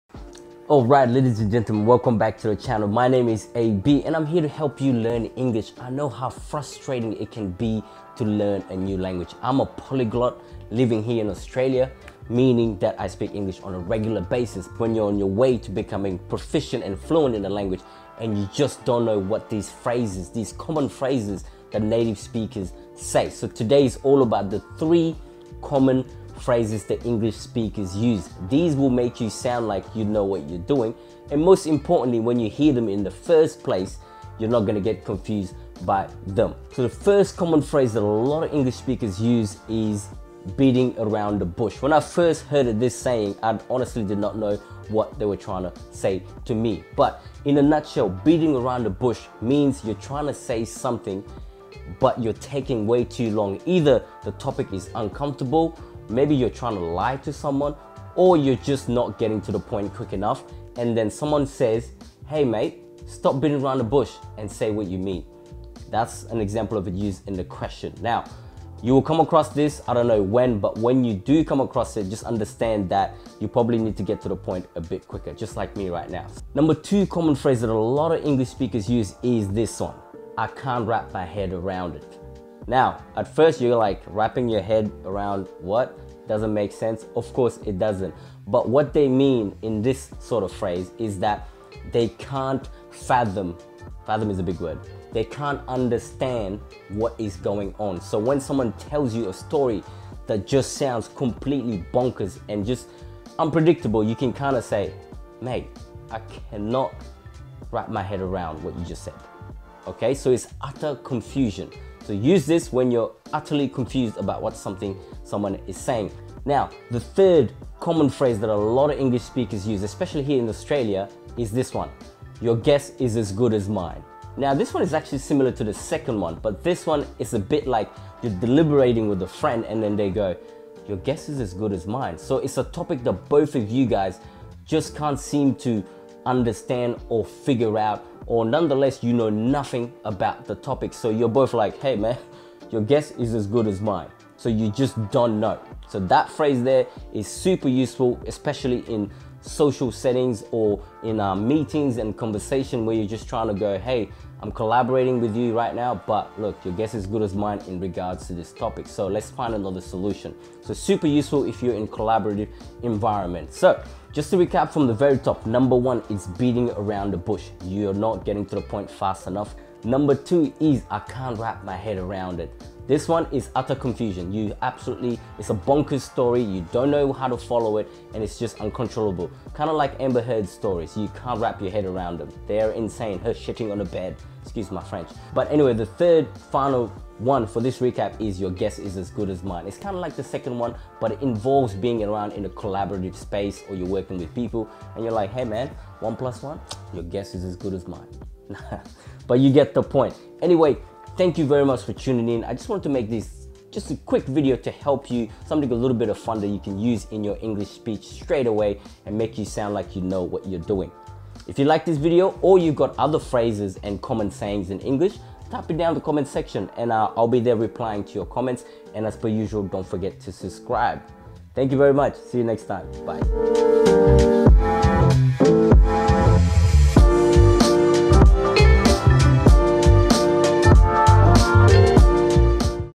When I was Learning English - These Sayings Confused Me
Hi all, I'm back again with another one! This time I thought we would take it easy and learn 3 common phrases English speakers use. These are super helpful and you can use them straight away!